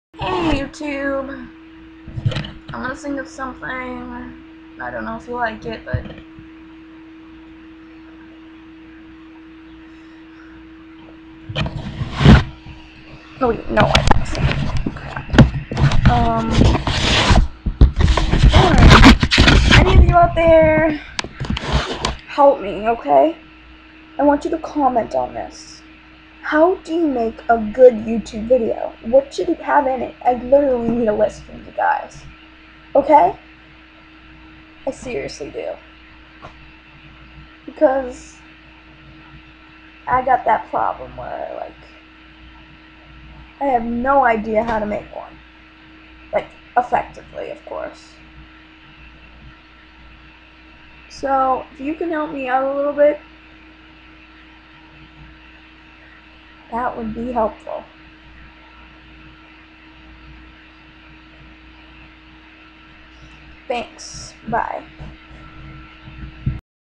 hOW DO YOU MAKE A GOOD VIDIO
i NEED SO MAGOR HELP HOW DO YOU MAKE A GOOD YOUTOUBE VIDIO PLESE TELL ME IIN YOUR COMMENTS BELOW